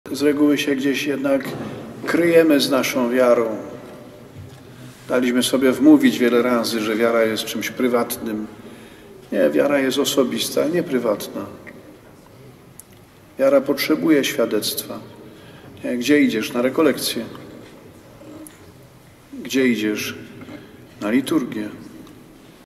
Wiara jest osobista, nie prywatna | abp Grzegorz Ryś
Wiara jest osobista, nie prywatna - mówił abp Grzegorz Ryś 3 kwietnia 2023 r. podczas Mszy świętej rozpoczynającej Wielkopostne Rekolekcje dla Łodzi.